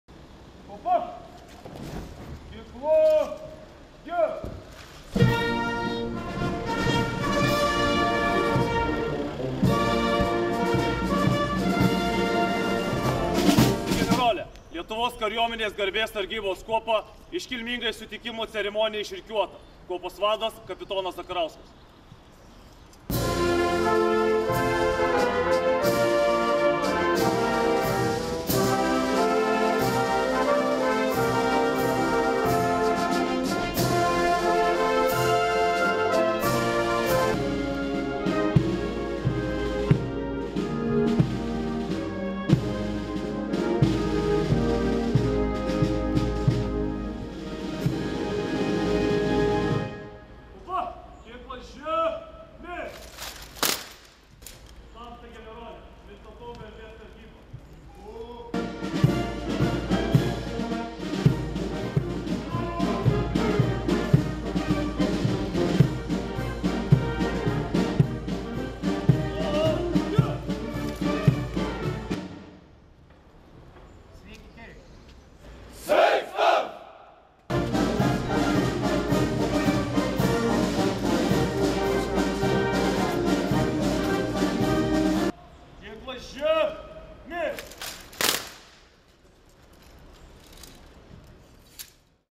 Lietuvoje lankosi Moldovos kariuomenės vadas
Birželio 7-9 d., Lietuvos kariuomenės vado kvietimu, su pirmuoju oficialiu vizitu Lietuvoje lankosi Moldovos kariuomenės vadas (Moldovos nacionalinės armijos Generalinio štabo viršininkas) brigados generolas Igor Cutie ir jį lydinti delegacija.
Aut. srž.sp. Lukas Tamošiūnas